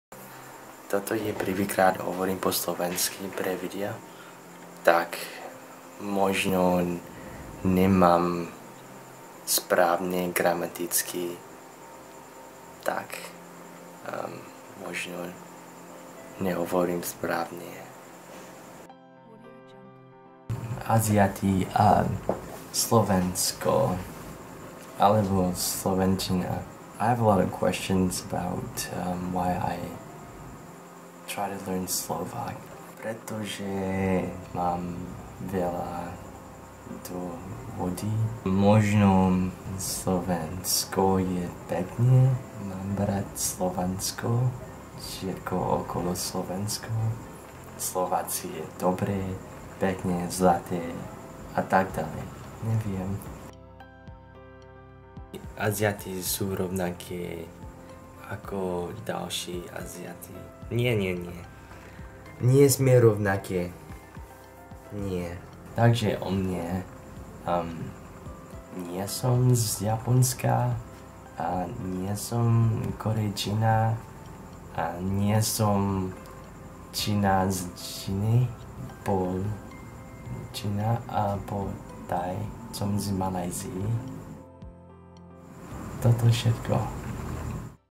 Asian learning Slovak
I try to speak in the best of my ability to sound decent but I am definitely all for native speakers to correct me. I've been learning on and off so I am not great at this. I'd be happy to hear constructive criticisms!